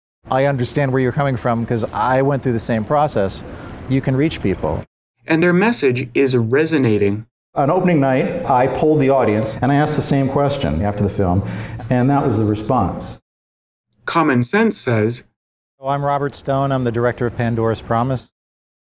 5h44m52s01f Robert Stone - You Can Reach People - Pandoras Promise - polled audience - TR2016a
I understand where you're coming from because I went through the same process. You can reach people.

And their message is resonating.

On opening night, I polled the audience, and I asked the same question after the film. And that was the response.

Common sense says...

I'm Robert Stone I'm the director of Pandora's Promise.